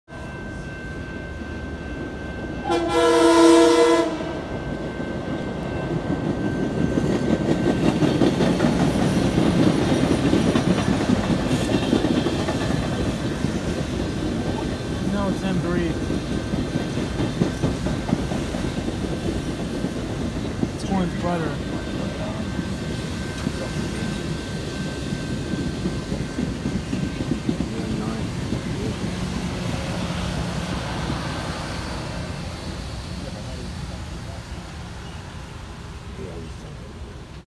M3 At Farmingdale
Taken on October 2019  I saw this m3 come by at Farmingdale